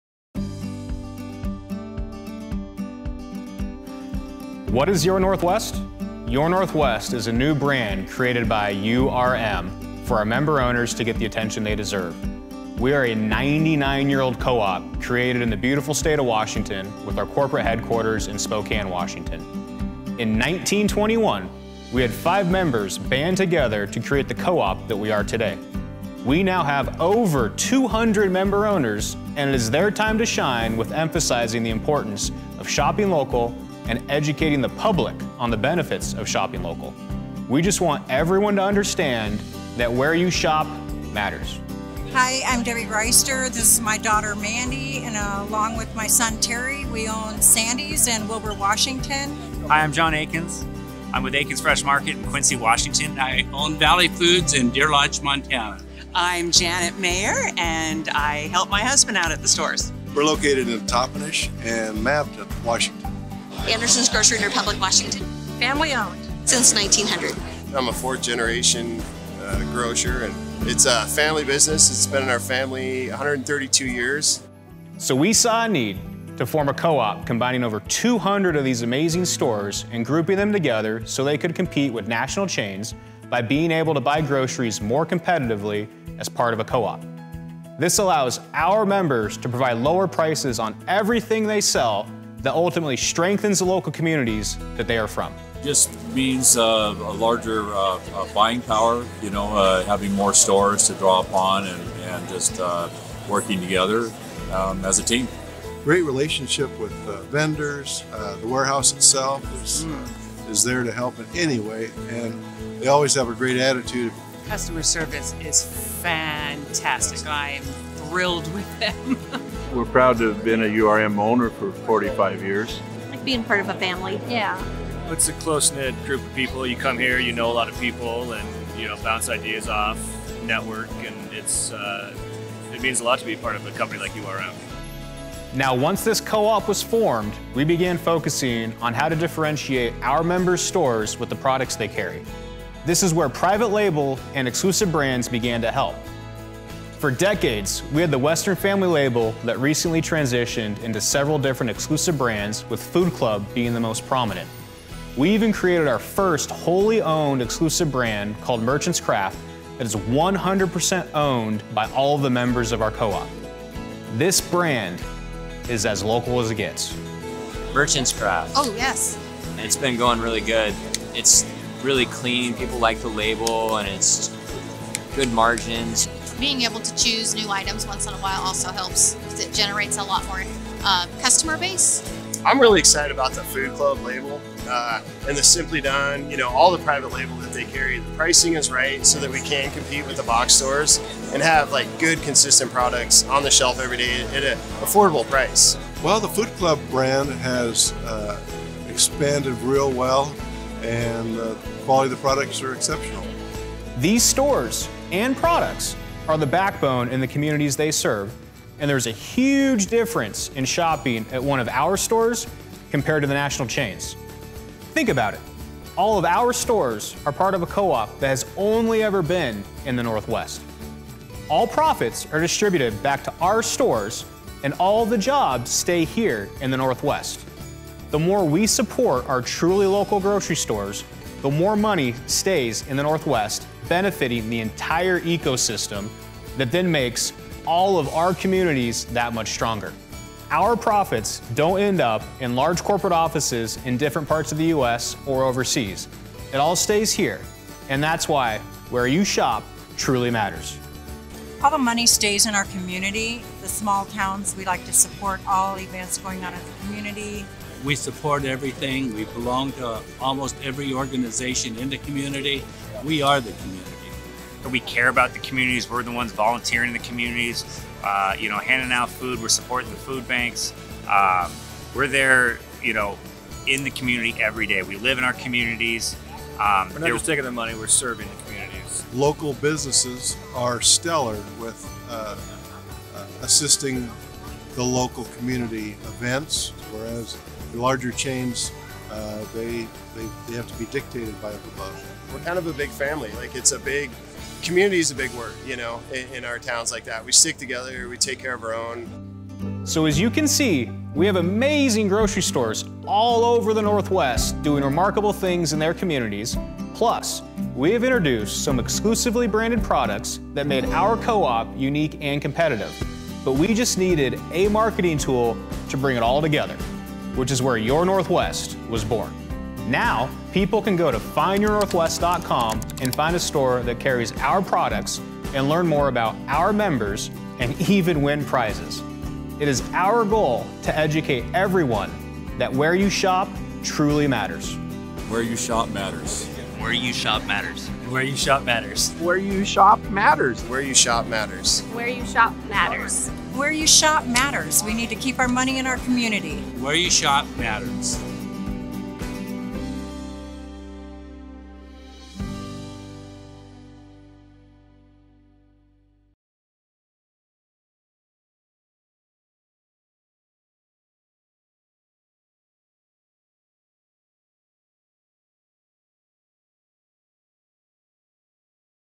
Your Northwest | Company Story Web Video
When it comes to grocery shopping in the Northwest, where you shop truly matters. Your Northwest was designed to give recognition to stores, big and small all across the Northwest including Washington, Idaho, Montana, and Oregon.